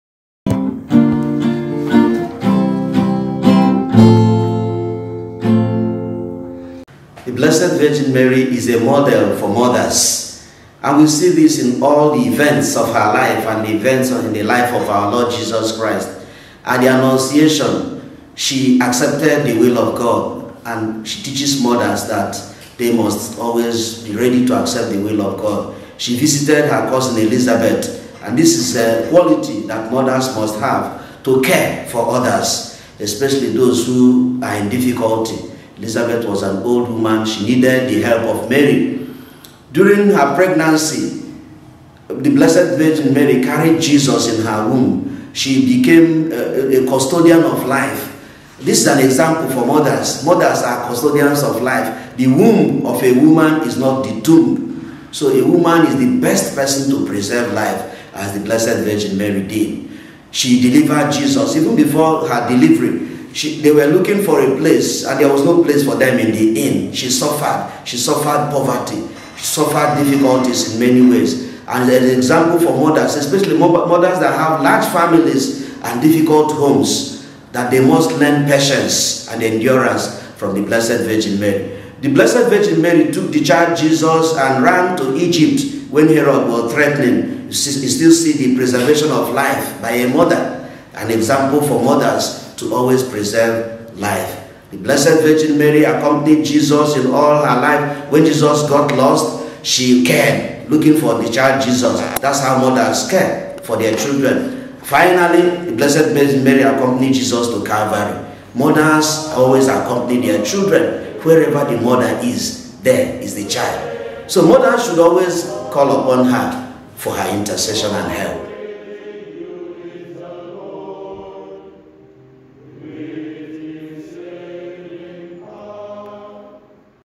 Mary, Model for Mothers by Fr. C. NNAMANI, OP
Despite her obvious fears and feelings of inadequacy, Mary saw her unexpected motherhood as a gift from God. She rejoiced that God had chosen her to give birth to his Son (Luke 1:47-49). She is a model for mothers.